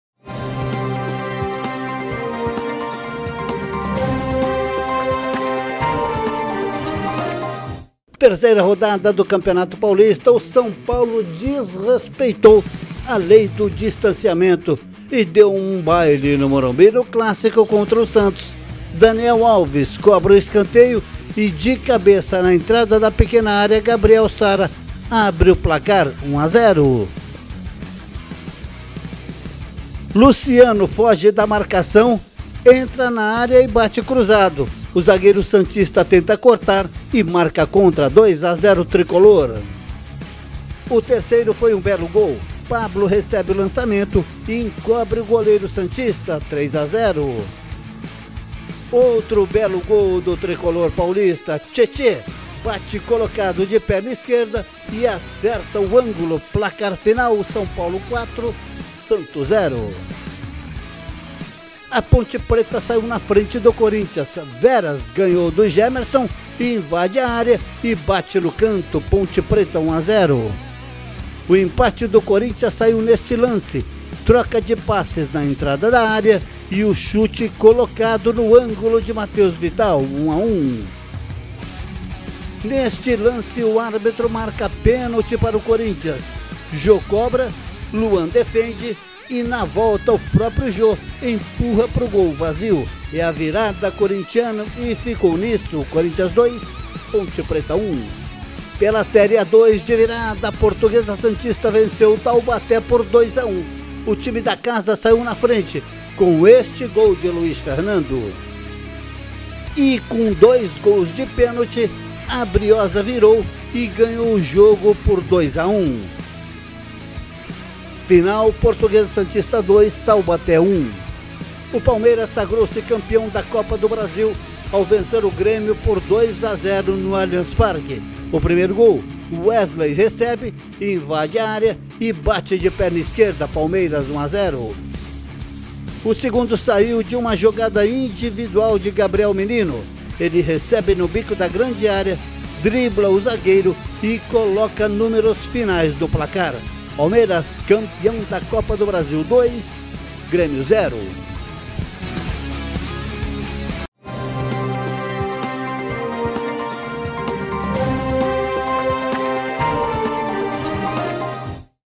CONFIRA OS GOLS DO PAULISTÃO E DA FINAL DA COPA DO BRASIL CONQUISTADA PELO PALMEIRAS
Pelo Paulistão, no clássico entre São Paulo e Santos deu tricolor, de lavada; Timão ganha da Ponte Preta, e Portuguesa Santista conquistou uma boa vitória fora de casa. Na finalíssima da Copa do Brasil, Palmeiras vence o Grêmio e conquista o tetra da competição.